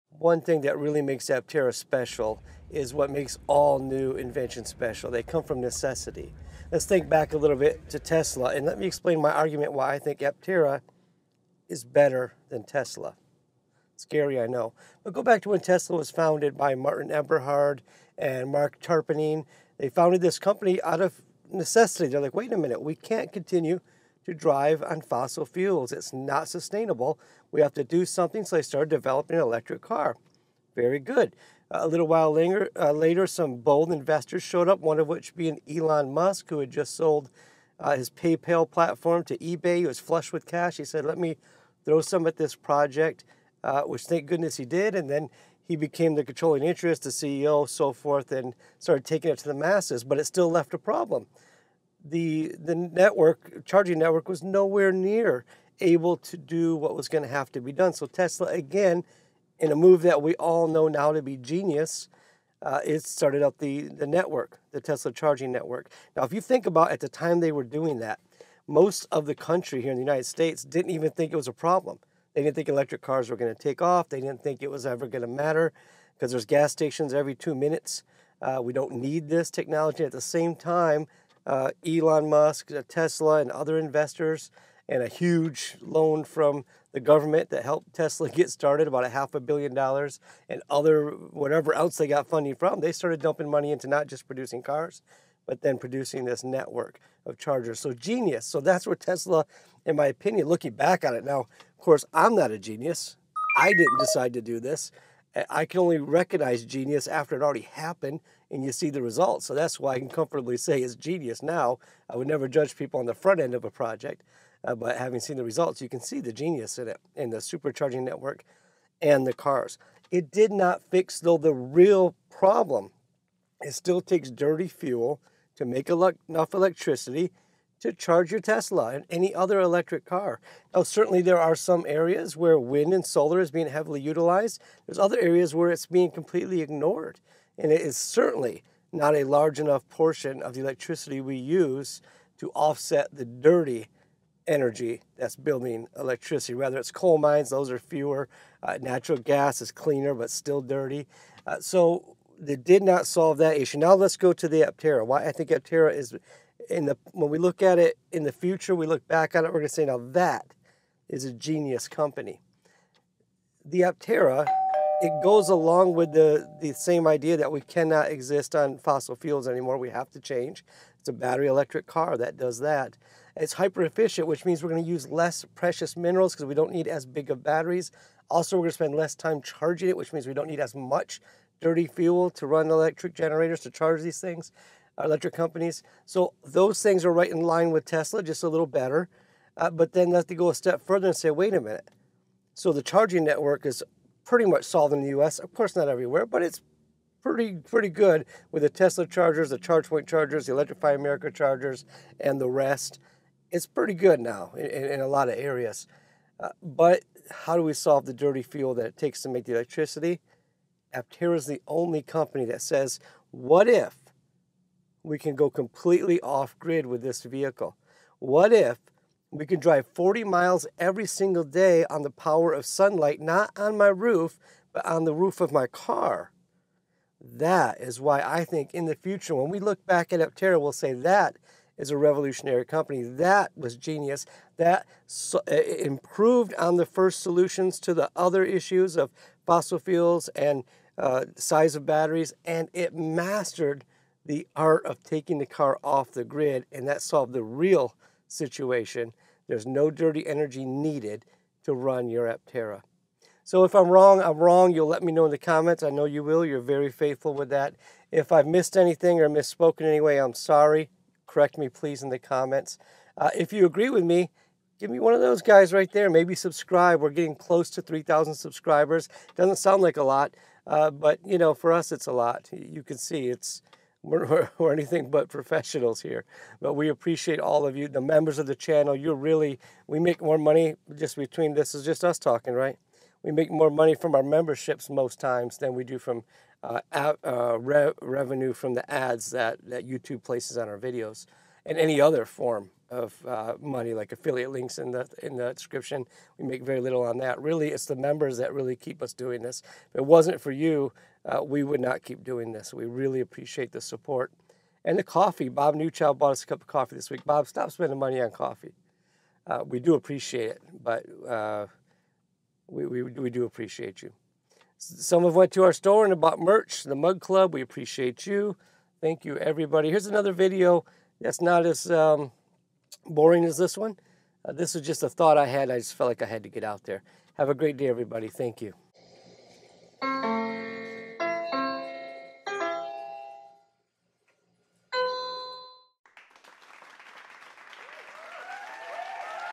Aptera Verse Tesla
Aptera Solar Electric Vehicle can be an Off the Grid daily driver!

DTL AMAZON ITEM OF THE MONTH
JULY 2024 - For the DTL Family Cats!

JUNE 2024 Affordable E-Bike!